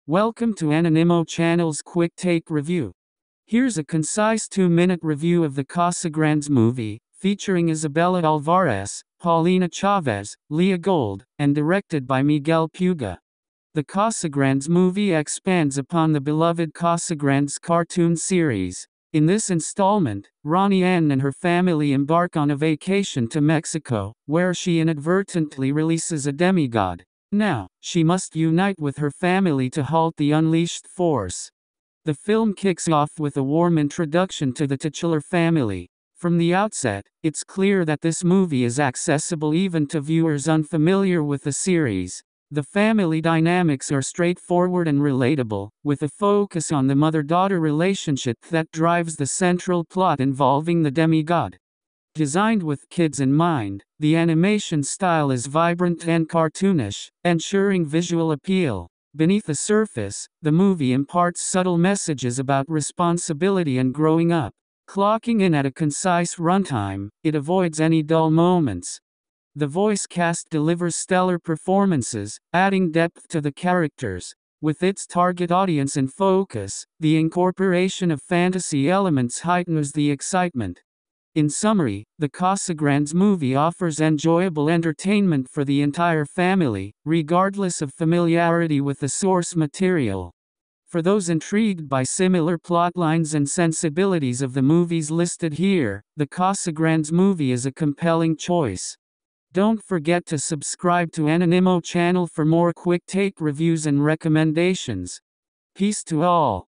The Casagrandes | Quick Take Movie Review
Welcome to Quick Take Movie Review Channel, where we bring you rapid-fire reviews of the latest movies! Today, we're diving into 'The Casagrandes Movie' in just 120 seconds. Join us as we break down the plot, performances, and overall impressions, giving you all the essential details in record time.